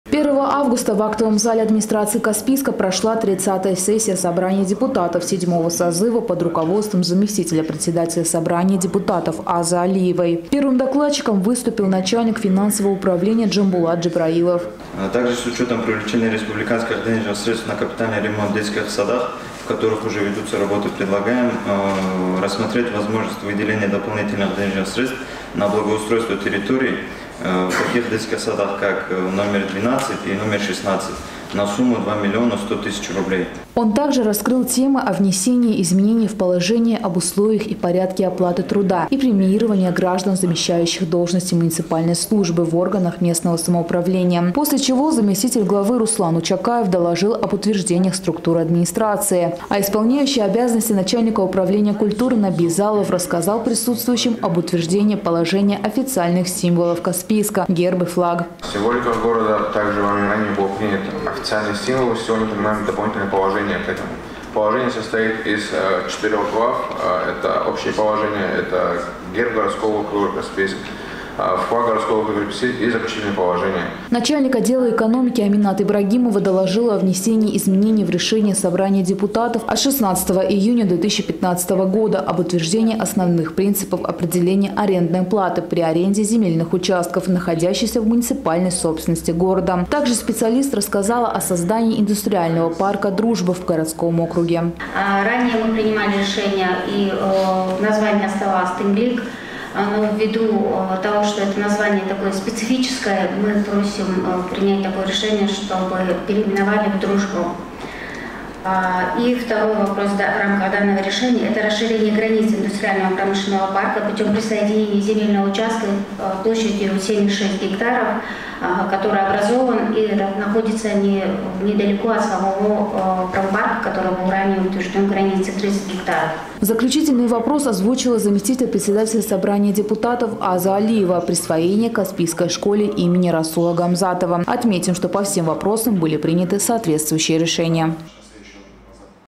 Бюджет Каспийска, создание индустриального парка «Дружба» и другие вопросы обсудили депутаты
1 августа в актовом зале администрации Каспийска прошла 30 сессия Собрания депутатов седьмого созыва под руководством заместителя председателя Собрания депутатов Азы Алиевой. На повестку дня были вынесены актуальные вопросы, первым докладчиком выступил начальник финансового управления Джамбулат Джабраилов.